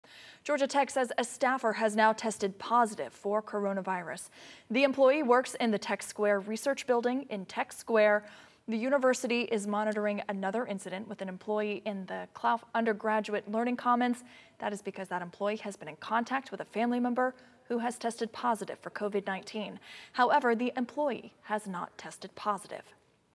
Employee at Tech Square tests positive for COVID-19
An employee who works in a building at Tech Square tested positive for COVID-19 according to a statement from Georgia Tech on Saturday.